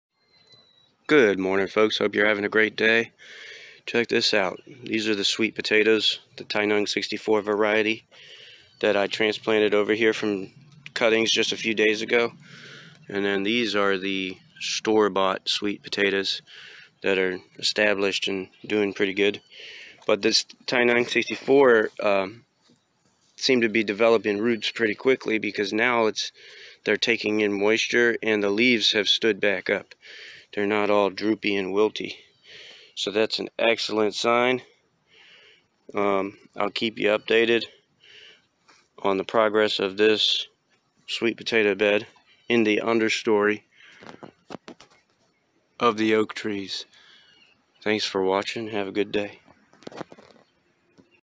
Sweet Potatoes In October
You are viewing the growth of sweet potatoes heading into October 2021 after propagation.  Watch video for details.  Thank you.